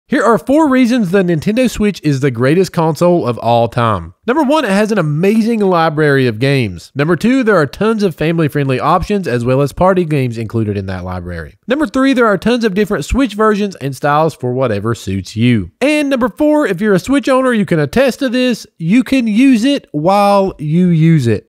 THIS is why the Nintendo Switch is the GREATEST Console of All Time
Be honest... the real reason the Nintendo Switch is the greatest console of all time is because you can play video games on the toilet. 🚽

This isn't just the only thing that makes it the greatest gaming system of all time, though.

What are some reasons you love the Switch? 🥳

🔦 RGB Tube Lights
🔦 RGB LED Lights
🪜 Light Stand
🎙 Mic | Shure SM7B
🔈Audio Interface
🖥 Computer | Cyberpower PC Pre-Built